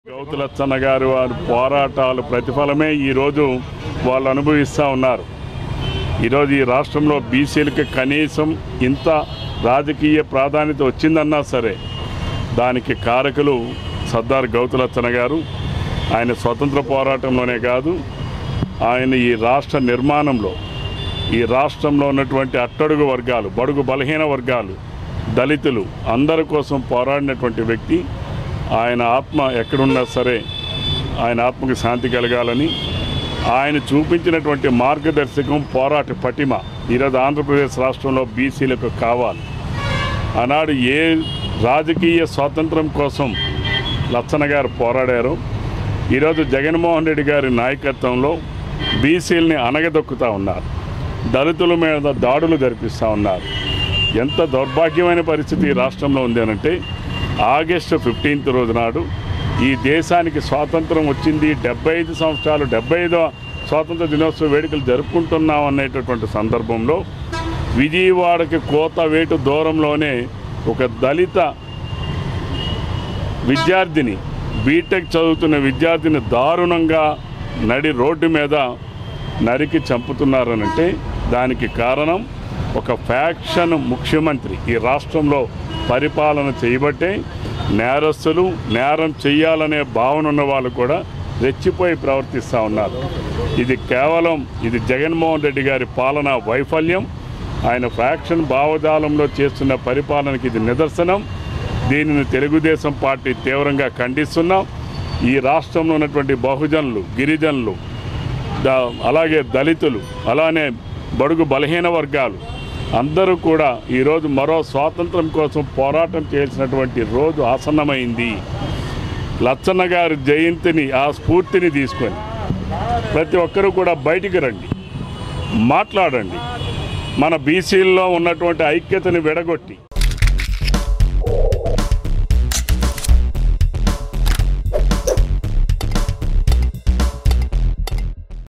సర్ధార్ గౌతు లచ్చన్న 112 వ జయంతి వేడుకలు | Sardar Gouthu Latchanna 112th Birth Anniversary
Sardar Gouthu Latchanna 112th Birth Anniversary: జిల్లా కేంద్రంలో సర్ధార్ గౌతు లచ్చన్న 112 వ జయంతి వేడుకలు..

డే అండ్ నైట్ కూడలి వద్ద గౌతు లచ్చన్న విగ్రహానికి పూలమాలలు వేస్ నివాళులు అర్పించిన టిడిపి నేతలు..

కార్యక్రమంలో పాల్గొన్న మాజీ ఎమ్మెల్యేలు కూనరవి, లక్ష్మీదేవి, టిడిపి రాష్ట్ర ప్రధాన కార్యదర్శి గౌతు శిరీష..

మాజీ ఎమ్మెల్యే కూనరవి కామెంట్స్..

ఆంధ్రప్రదేశ్ రాష్ట్ర నిర్మాణంలో సర్దార్ గౌతు లచ్చన్న పాత్ర చాలా కీలకమైనది..

గౌతు లచ్చన్న చరిత్రలో నిలిచిపోయే నాయకుడు..

రాష్ట్రంలో బీసీలకు రాజకీయ అధికారం వచ్చిందంటే లచ్చన్న వంటి మహనీయుల పోరాట ప్రతిఫలమే..

పలాసలో ఉండే అప్పలరాజుకి మంత్రి పదవి దక్కింది అంటే కూడా అదే కారణం..

బీసీల విషయంలో న్యాయం కోసం లచ్చన్న వంటి నాయకులు పోరాటాలు చేశారు..

కానీ జగన్ ప్రభుత్వంలో బీసీలను అణగదొక్కుతున్నారు..

రాష్ట్రంలో దౌర్భాగ్య పరిస్థితులు నెలకొన్నాయి..

75 ఏళ్ళ దేశ స్వాతంత్ర్య దినోత్సవ వేడుకలు జరుపుకుంటున్న రోజునే దళిత విద్యార్ధినిని నడిరోడ్డు మీద హత్యకు గురికాబడింది..

ఫ్యాక్షన్ భావజాలం కలిగిన ముఖ్యమంత్రి పరిపాలన చేయబట్టే రాష్ట్రంలో నేరస్తులు రెచ్చిపోయి ప్రవర్తిస్తున్నారు..

ఇది కేవలం జగన్ పాలనా వైఫల్యం.. జగన్ ఫ్యాక్షన్ భావజాలానికి నిదర్శనం..

ప్రస్తుత పరిస్థితుల్లో బడుగుబాలహీన వర్గాలు , బాహుజనులు, గిరిజనులు, దళితులు అంతా మరో స్వాతంత్ర్యం కోసం పోరాటం చేయాల్సిన సమయం ఆసన్నమైంది..

మాట్లాడితే 56 కార్పొరేషన్ లు ఏర్పాటు చేశాం అని జగన్ చెప్పుకుంటున్నాడు..

అసలు బడ్జెట్ లో ఈ 56 కులాల అభ్యున్నతి కోసం కేటాయించిన నిధులు ఏమిటి ? వారికి ఇచ్చిన విధులు ఏమిటి జగన్ చెప్పాలి..

స్థానిక సంస్థల్లో బీసీలకు 33 శాతం ఉన్న రిజర్వేషన్ ని 24 శాతానికి కుదించిన ఘనత జగన్ ది..

బీసీలకు రాజకీయ అధికారం కూడా లేకుండా చేయాలని జగన్ కుట్రపన్నుతున్నాడు..

రాష్ట్రంలో రాజారెడ్డి రాజ్యాంగాన్ని అమలు చేస్తూ జగన్ బీసీల ఐక్యతను దెబ్బతీసే ప్రయత్నాలు చేస్తున్నాడు